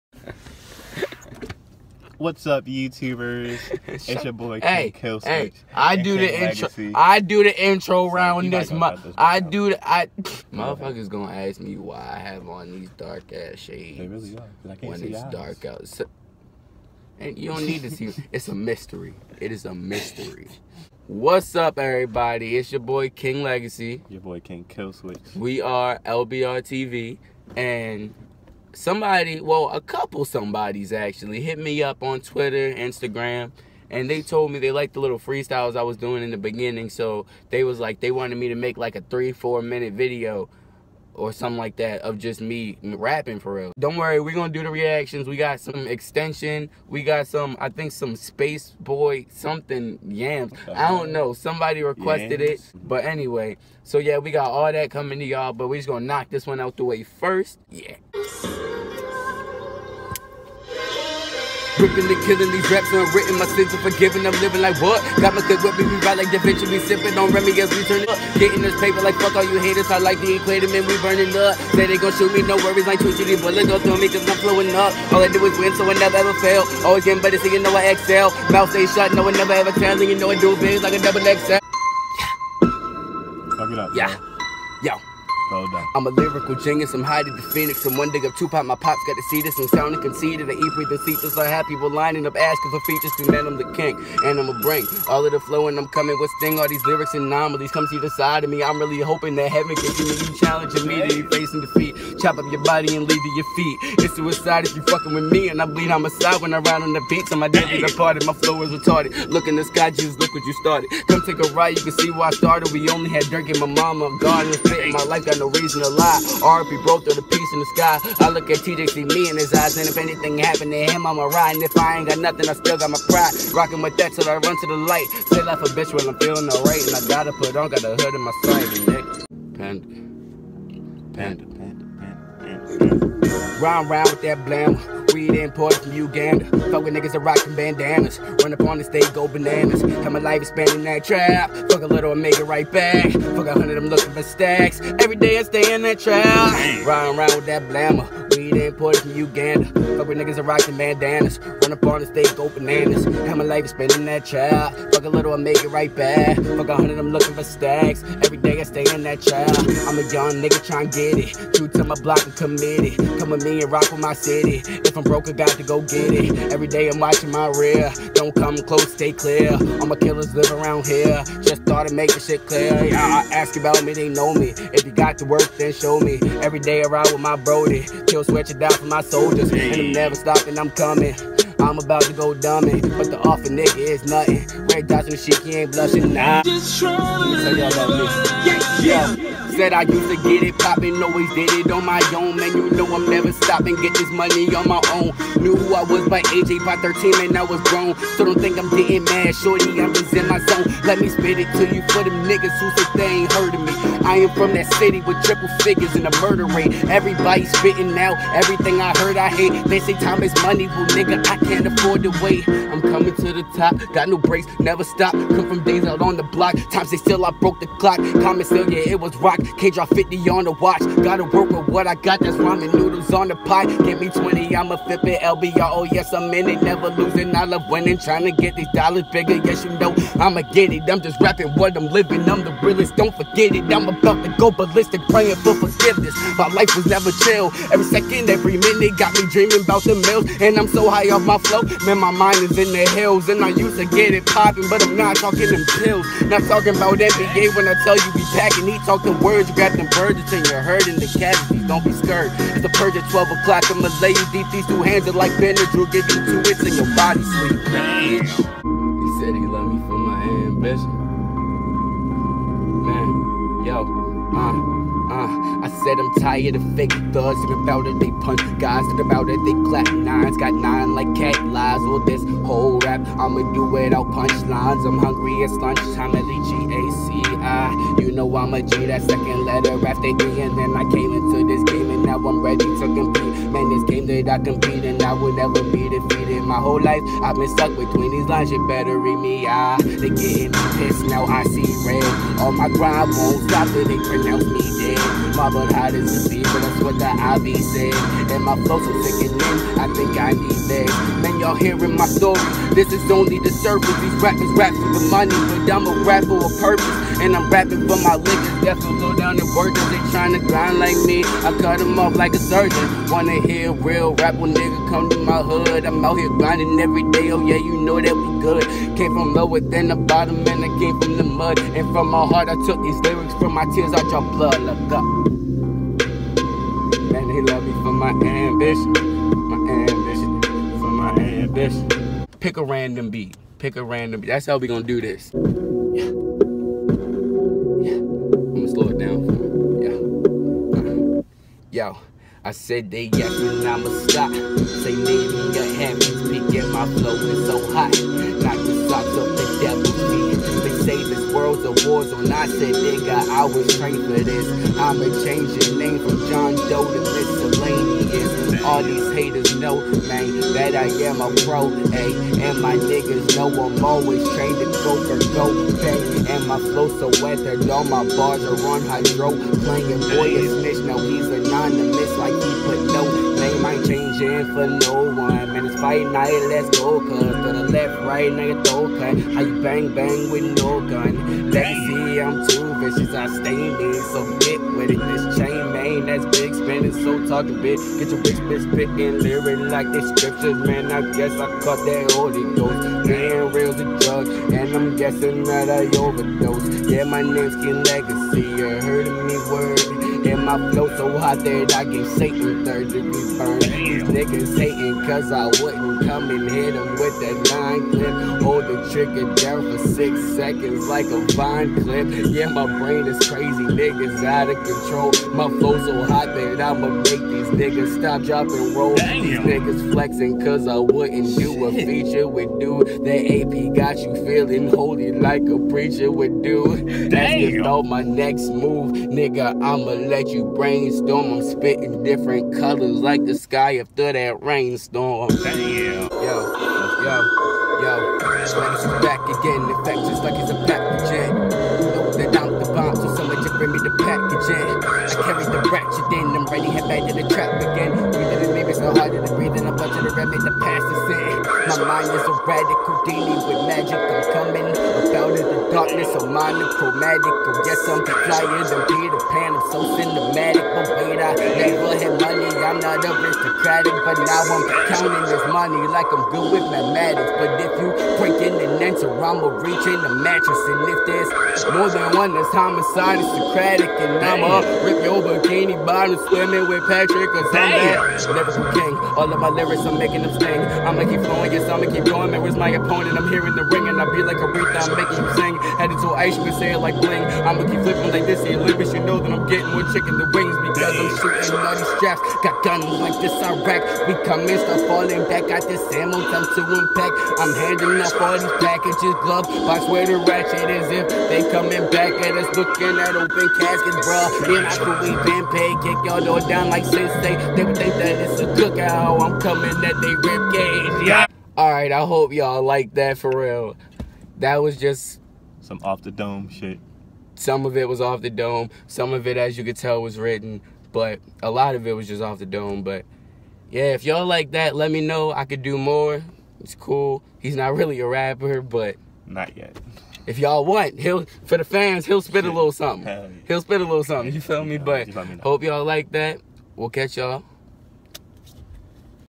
KING LEGACII 9 MINUTES OF BARS !!!
Was Requested To Do A 3 Or 4 Minute Rapping video. As Y'all Can't see i got A little Carried Away lol

Follow Me Legacii On Twitter And Ig

Follow Killswitch King On Ig